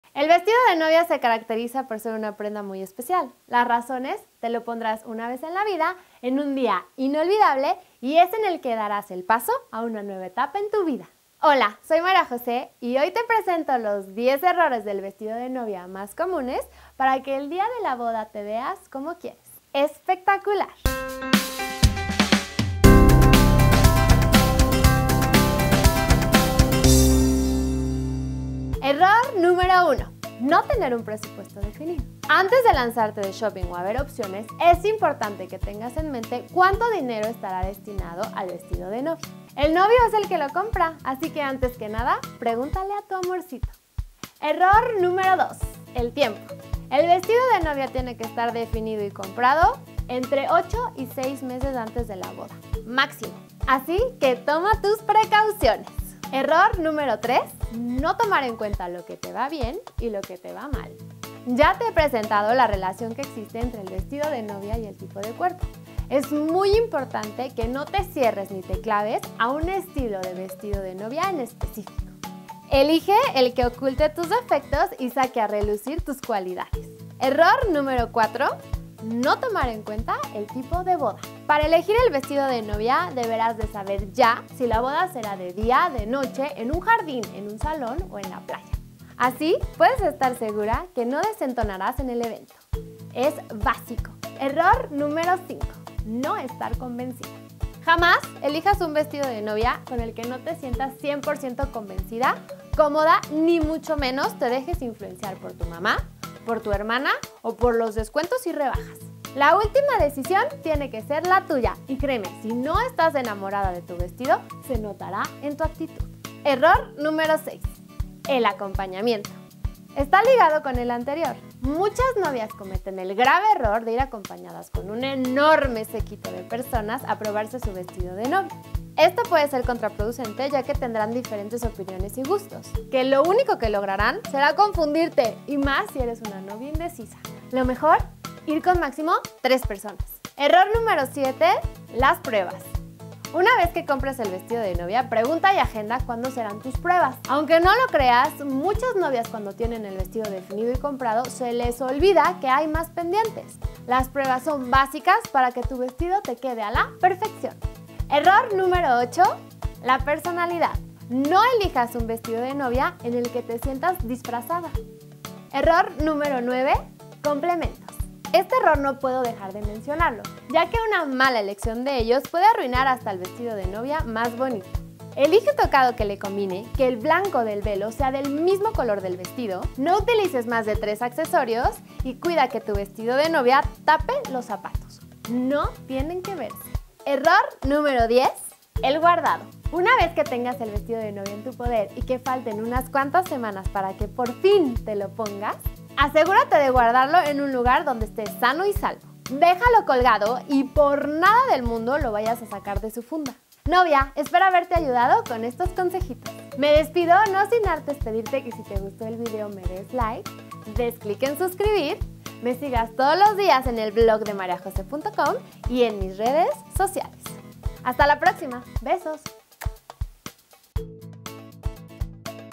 Errores del vestido de novia - El Blog de María José
El vestido de novia es una prenda muy importante ya que se utiliza en un día muy especial y una vez en la vida, por lo que toda mujer que está con planes de boda quiere lucir perfecta en él. Te presento los diez errores más comunes del vestido de novia para que logres tu objetivo.

¿Te casas? Te presento ideas, consejos y tendencias para organizar la boda de tus sueños.